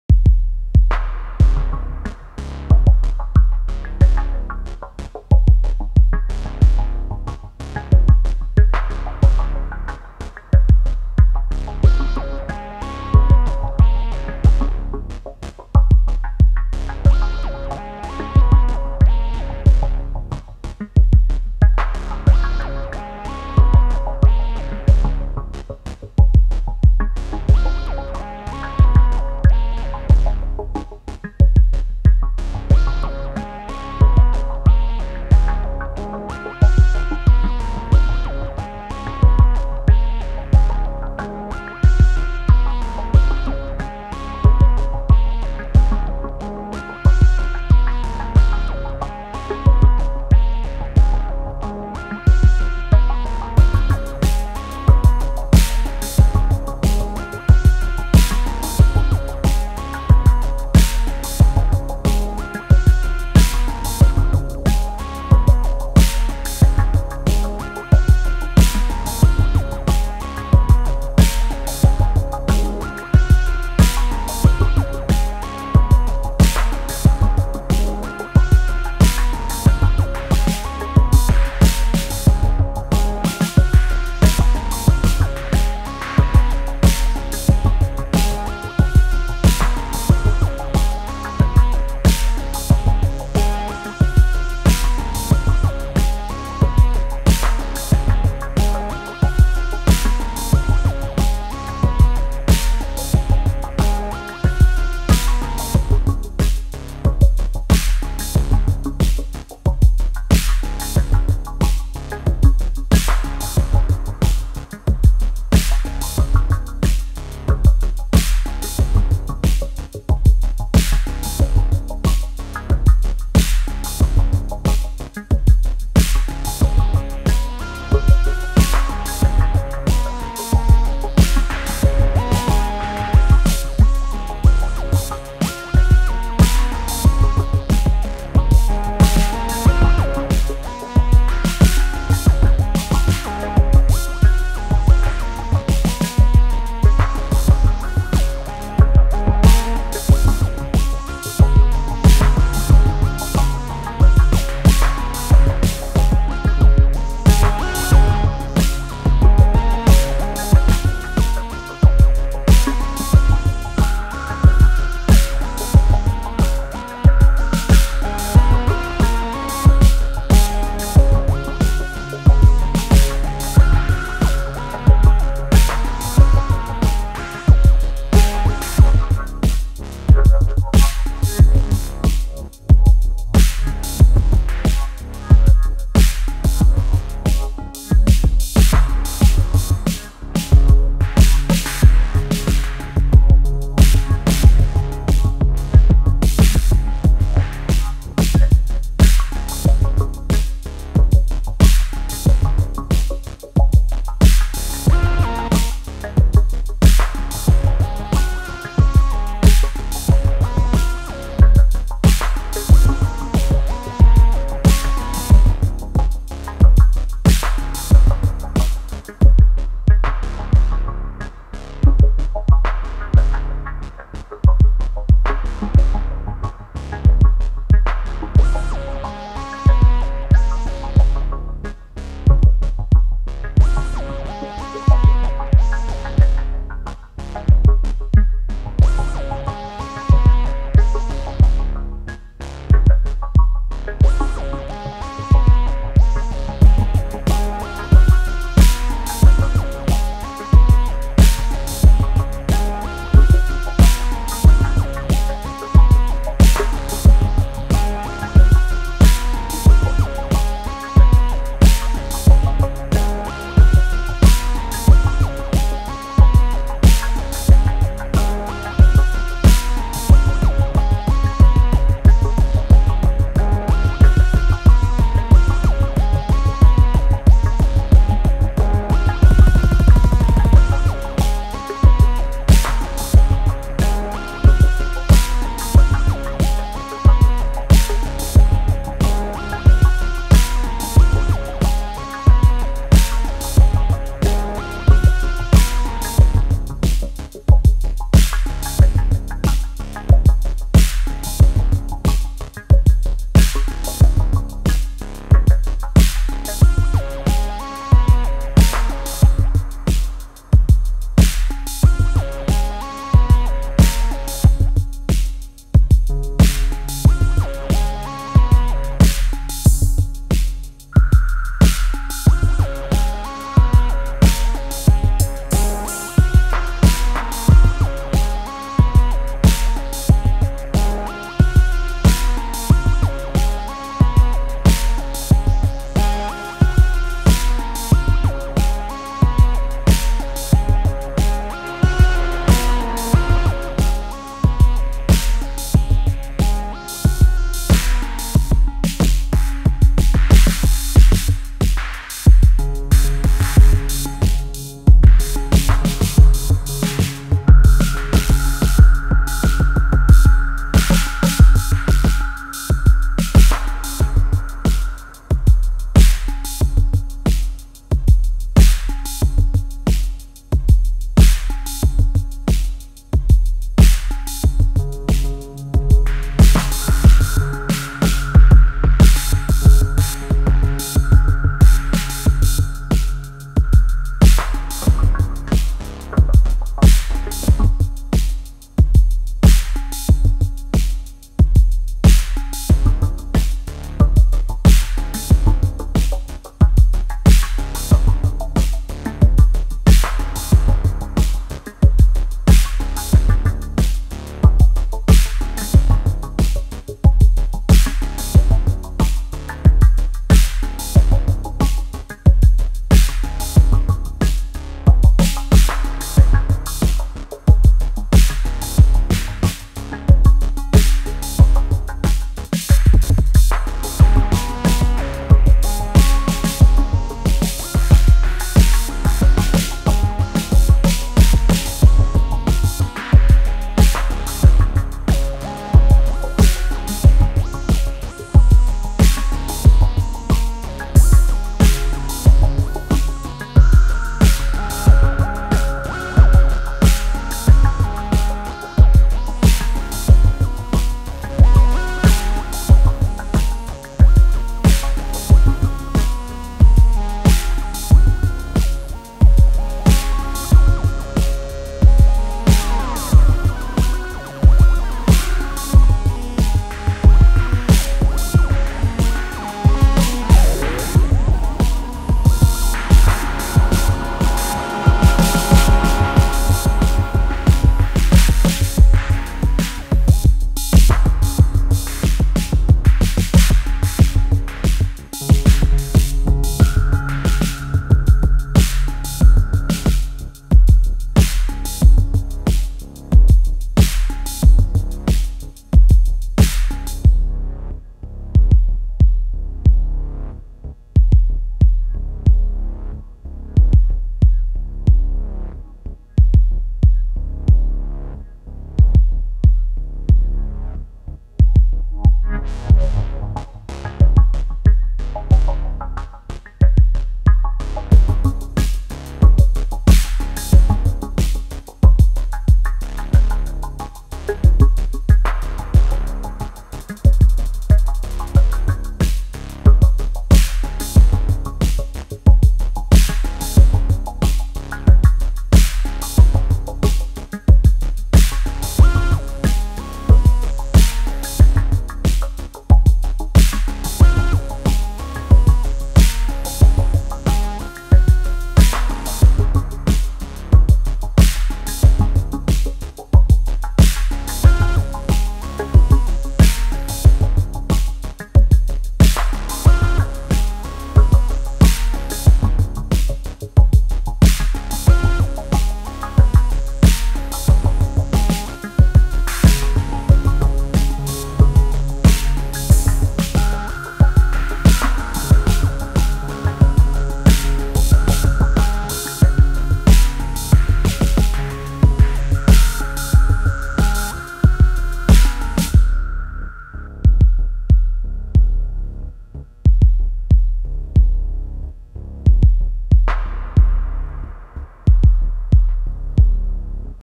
OMIEM Sketch01 29 22
Recently picked up a RolandBoutiqueSE-02 and a ArturiaDrumbruteImpact and had some fun sketching over a AlesisMicron loop and layering in with ElectronModelSamples